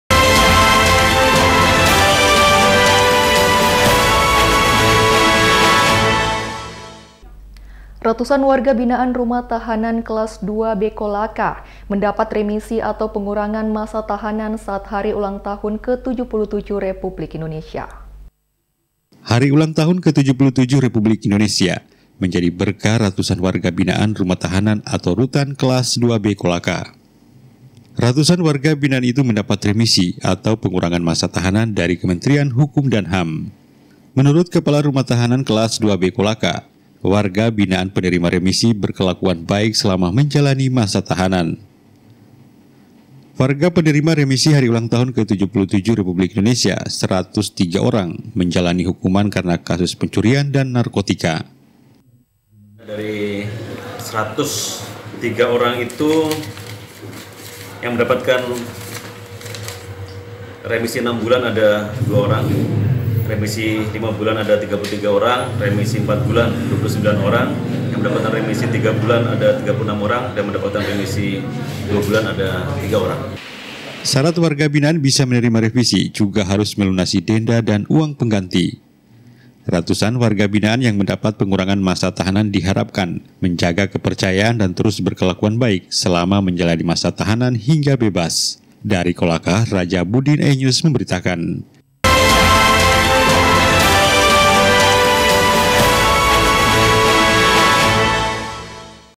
Ratusan Warga Binaan Rutan Kolaka Terima Remisi HUT ke-77 RI
iNews Sultra, 19 Agustus 2022